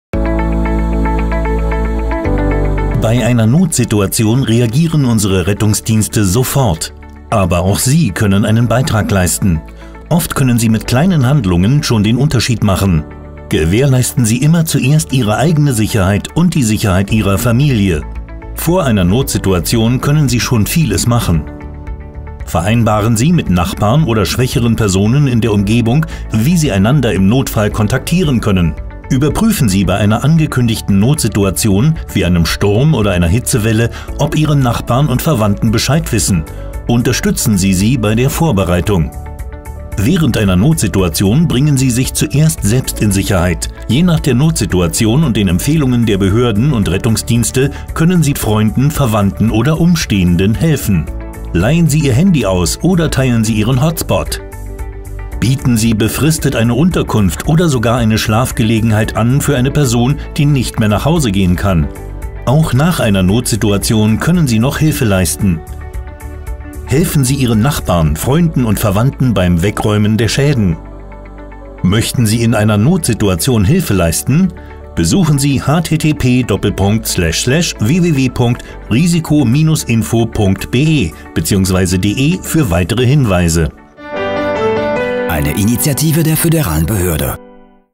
Zusammen zeigen wir uns in einer Notsituation solidarisch
Im Notfall reagieren die Rettungsdienste sofort, um die Situation möglichst schnell unter Kontrolle zu bringen und die Folgen zu beschränken. Aber auch Sie können einen Beitrag leisten.

Indem Sie vor, während oder nach einer Notsituation die Ärmel hochkrempeln, erleichtern Sie die Arbeitsbelastung der Polizei, der Krankenwagen oder der Feuerwehr, die auf diese Weise die dringendsten Opfer zuerst behandeln können. Oft können Sie mit kleinen Handlungen schon den Unterschied machen.